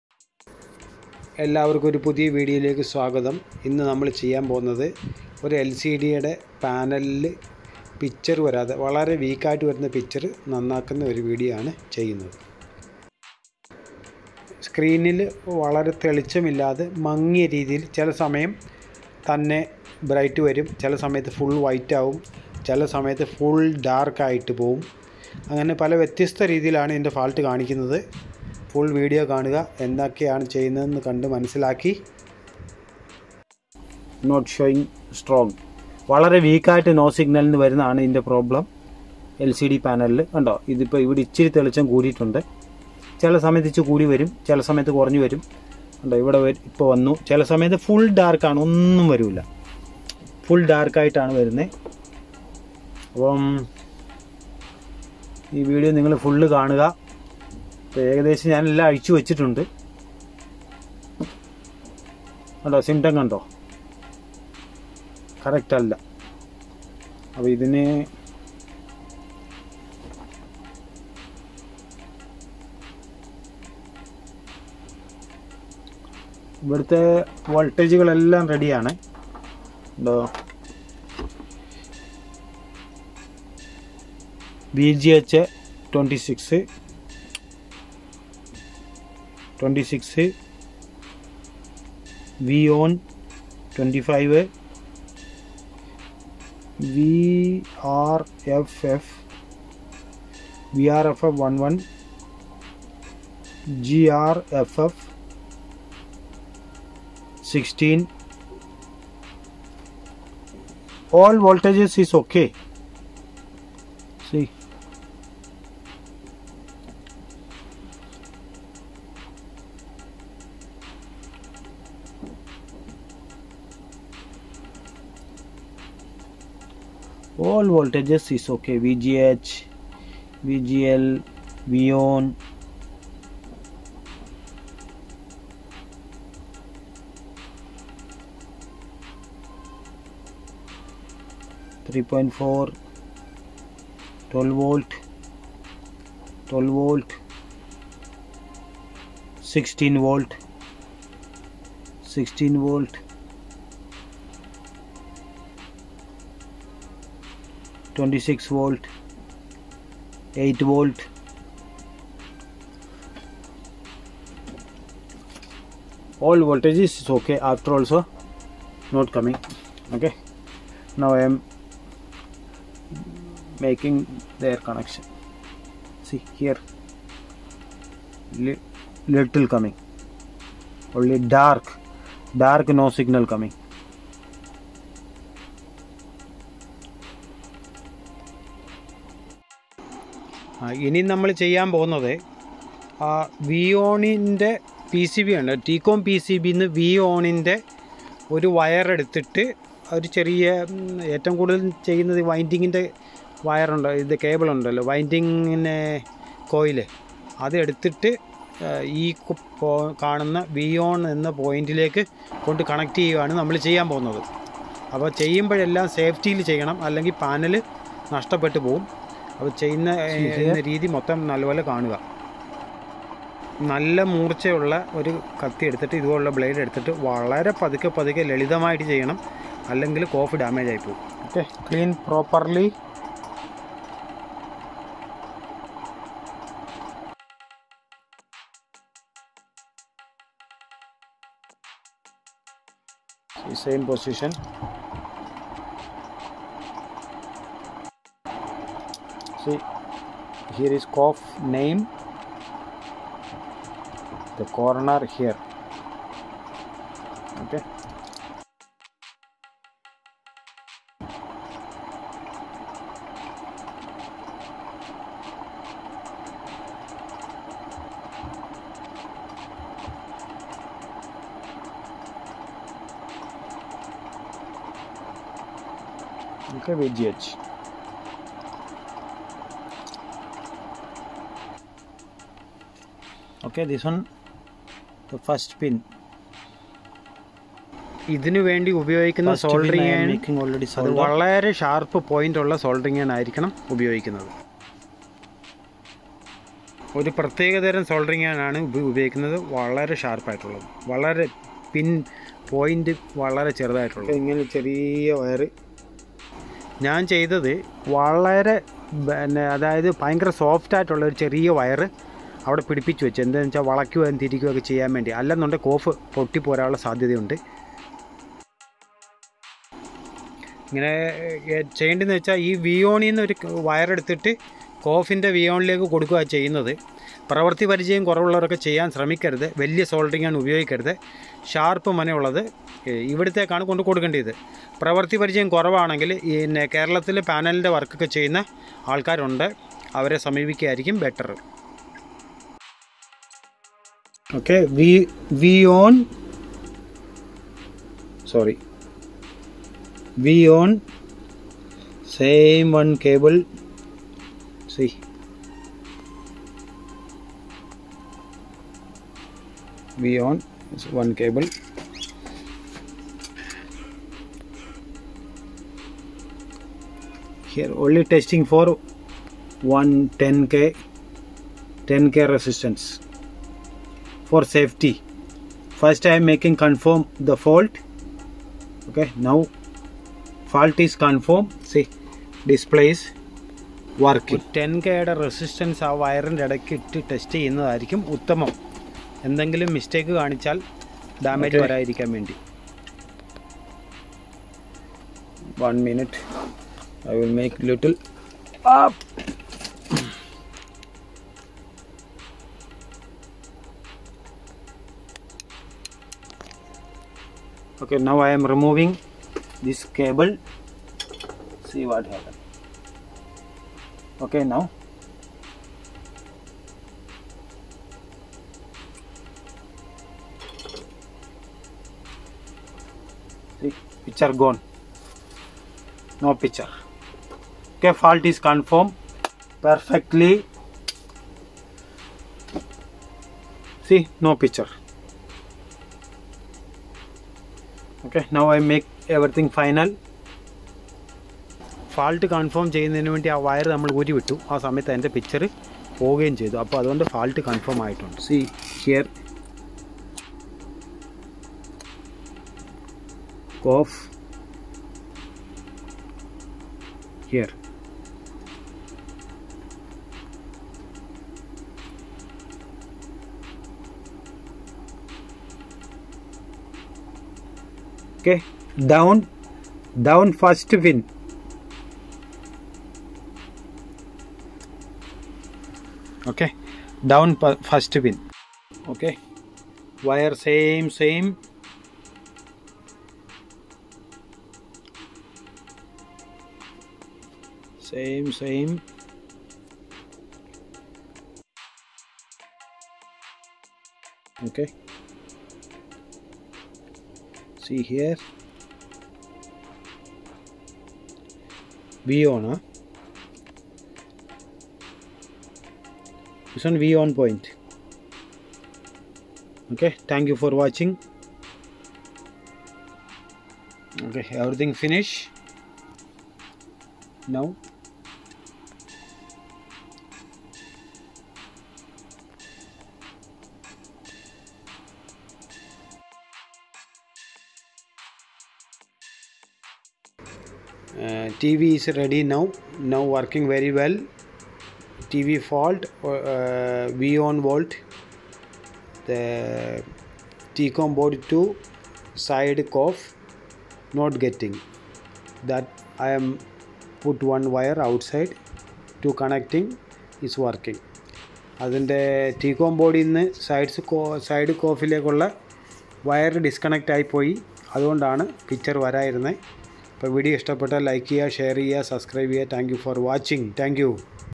LCD panel repairing. 8656F,. HV320WXC-200-X
LCD panel repairing video . LG TV no picture. coff8656 f. hv329wxc-200-xpcb-x0.0, എൽസിഡി പാനൽ റിപ്പയറിങ് വീഡിയോ മലയാള. ടി വി റിപ്പയറിങ് മലയാളം വീഡിയോ